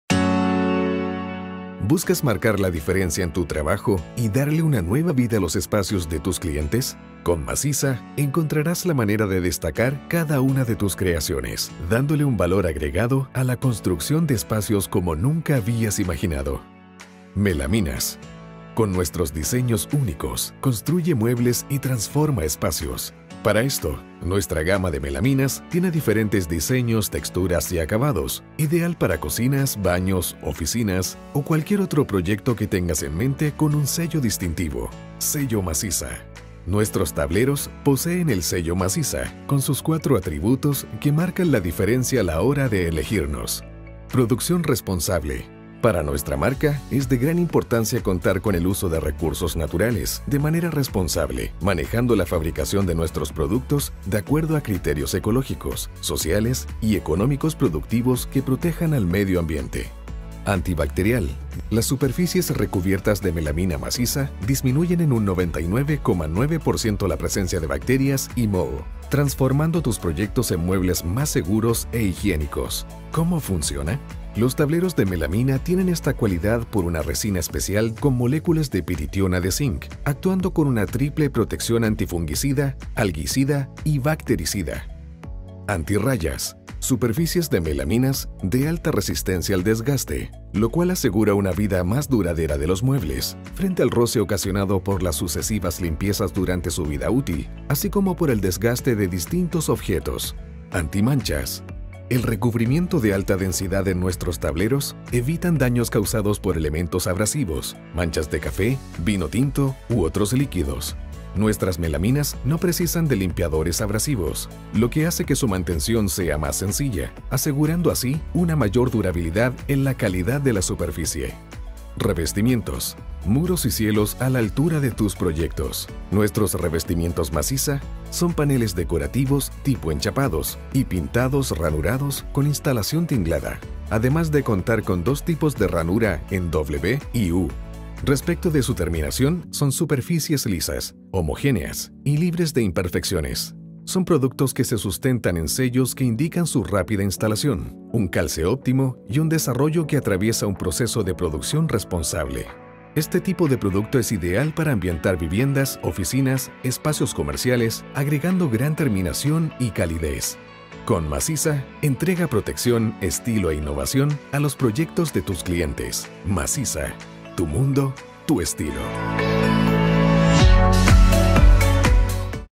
Conoce los 4 sellos que Masisa tiene para tu próximo proyecto
"La calidad de los productos Masisa está presente a través de sus sellos brindando seguridad y durabilidad a todas sus soluciones. Conoce los 4 sellos que Masisa tiene para tu próximo proyecto"